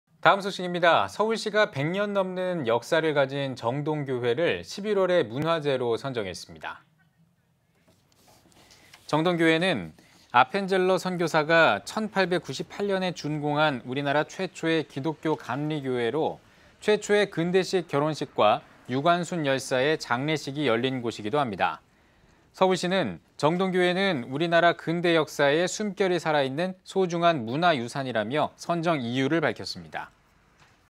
36년 역사 정동교회, 서울시 지정 11월 문화재 선정 (이한승) l CTS뉴스
서울시가 100년이 넘는 역사를 가진 정동교회를 11월의 문화재로 선정했습니다.

정동교회는 아펜젤러 선교사가 1898년 준공한 우리나라 최초의 기독교 감리교회로 최초의 근대식 결혼식과 유관순 열사의 장례식이 열린 곳이기도 합니다.

서울시는 “정동교회는 우리나라의 근대역사의 숨결이 살아있는 소중한 문화유산”이라며 선정 이유를 밝혔습니다.